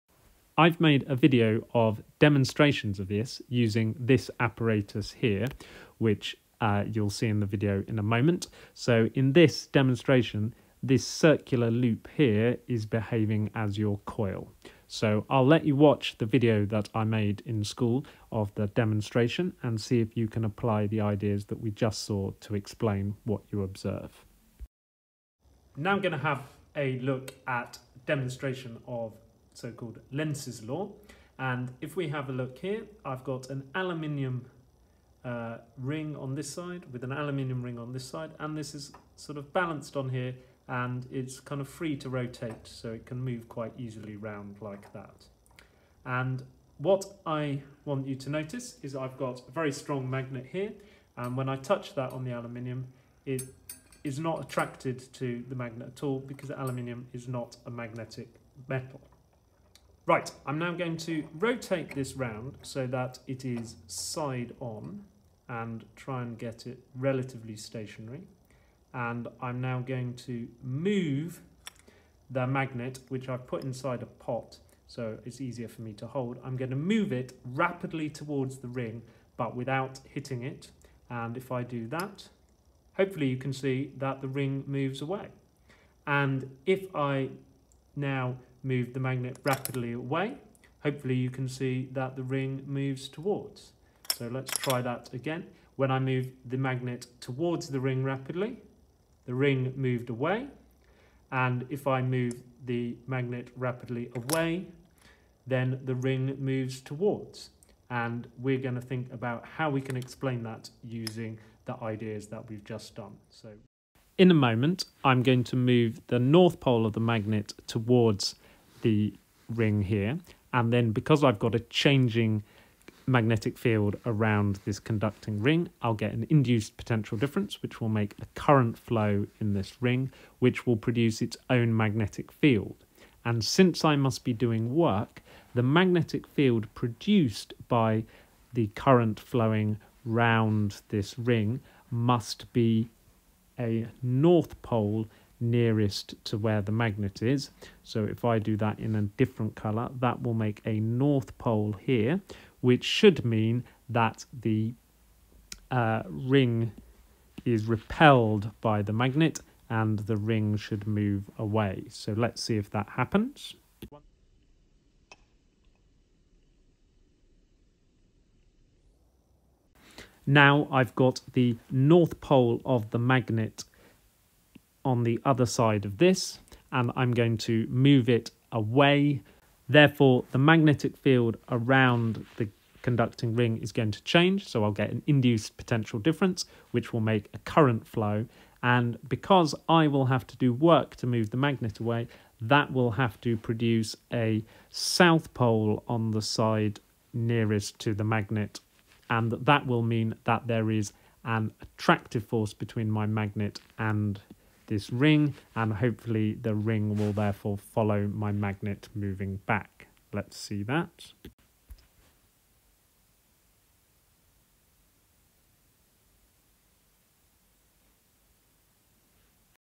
Simple demonstration of Lenz’s law
Suitable demonstration for teaching the idea of Lenz's law at GCSE or A-level Physics.